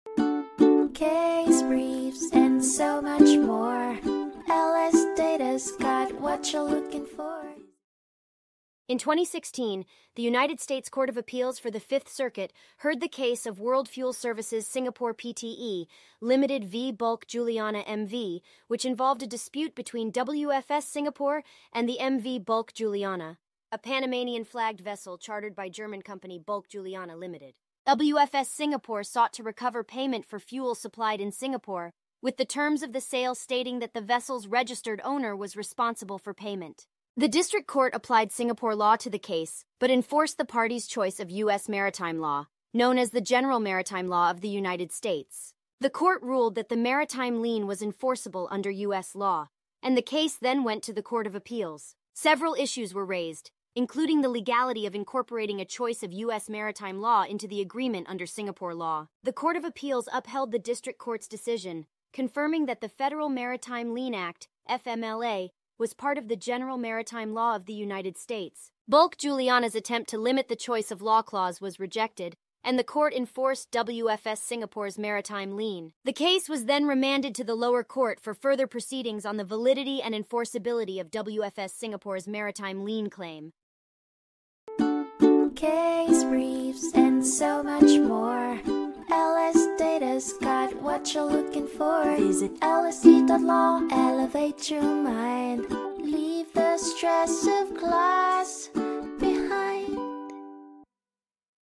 World Fuel Services Singapore Pte, Ltd. v. Bulk Juliana M/V (2016) Overview | LSData Case Brief Vide
WFS Singapore is seeking payment for fuel delivered to a US-owned vessel chartered by a German company called Bulk Juliana. The district court applied Singapore law and enforced the parties' choice of law as the "General Maritime law of the United States." The district court granted summary judgment and ruled that the choice-of-law provision was valid under Singapore law, and U.S. law applied, making the maritime lien enforceable under the federal maritime lien statute. The appeal raises several issues, including the validity of incorporating a choice of U.S. maritime law into the agreement under Singapore law.

World Fuel Services Singapore Pte, Ltd. v. Bulk Juliana M/V (2016)
822 F.3d 766